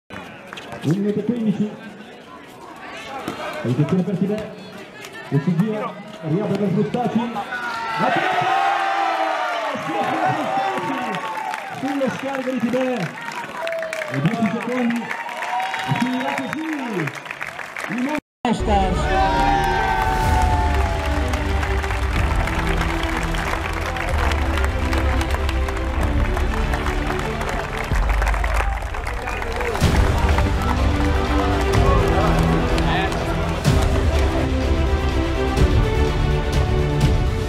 [Binzago Moments] #230 - La tripla di Frustaci mette il sigillo: Monstars 2° titolo (2023)
2023 - La tripla di Sofia Frustaci, su scarico di Angelica Tibè, è il colpo decisivo nel break che regala ai Monstars il 2° titolo di Binzago dopo una durissima battaglia con i Fuskugnaskamukula.
Torneo di Binzago, 16/7/23